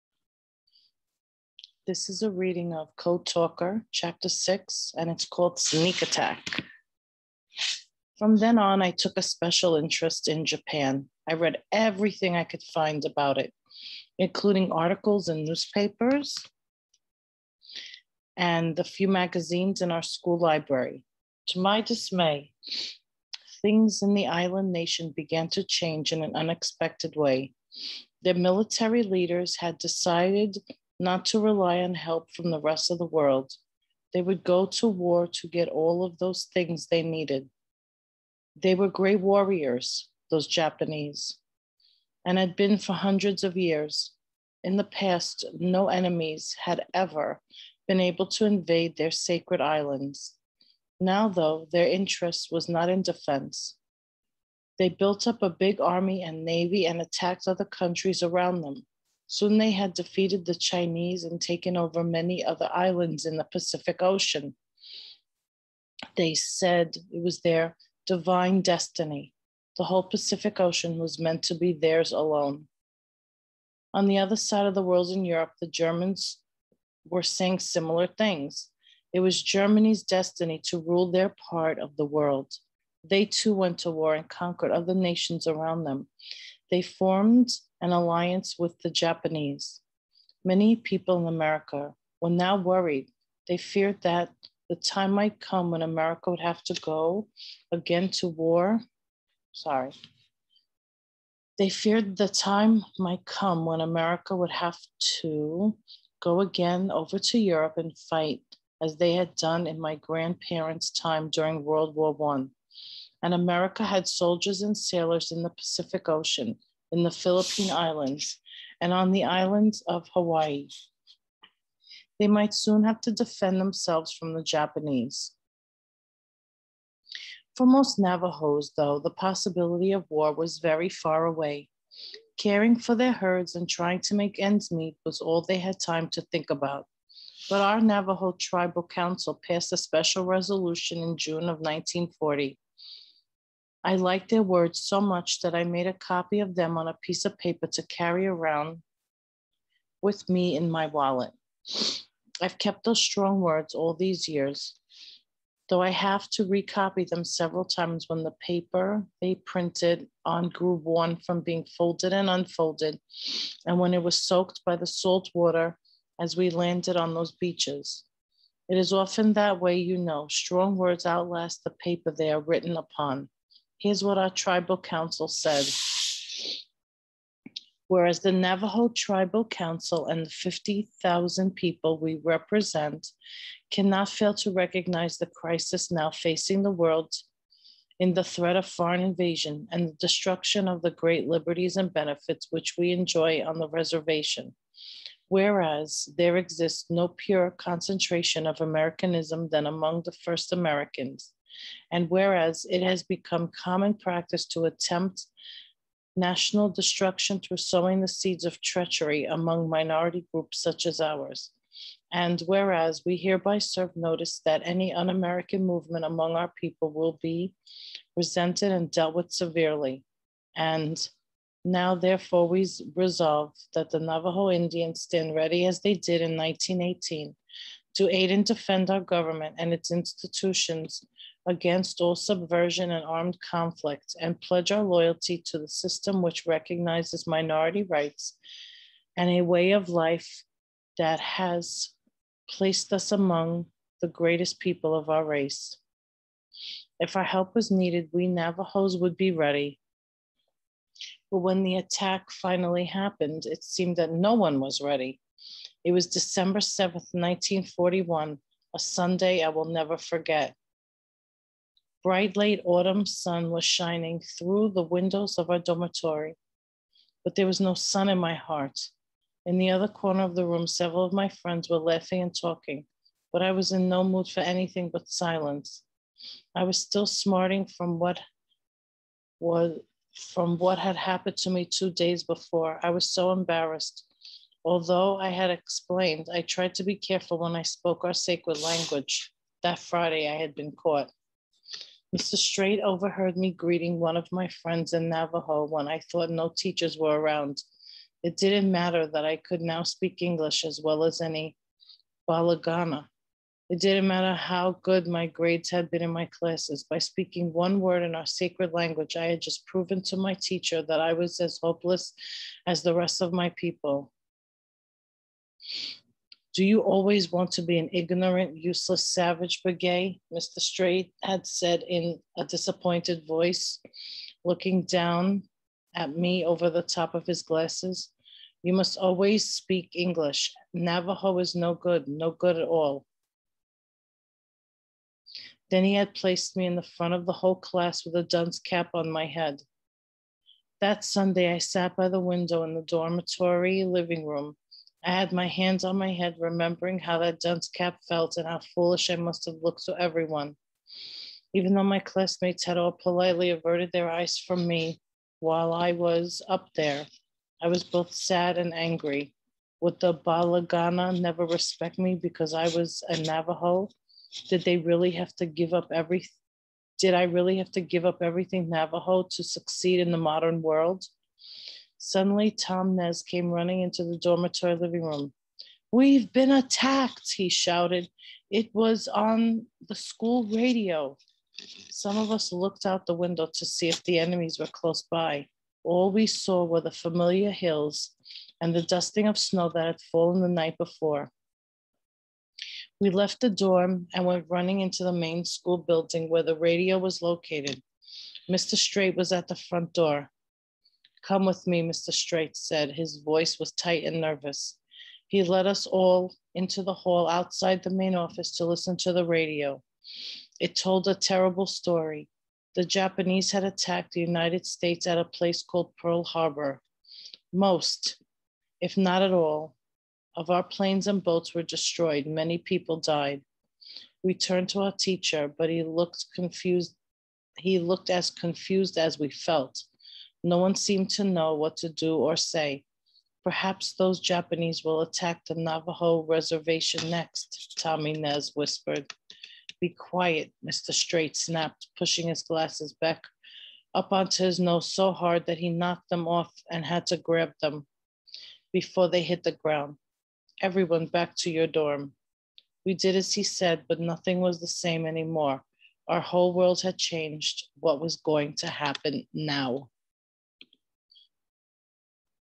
Code Talker by Joseph Bruchac - Chapter 6
This video is a read aloud with the text shown.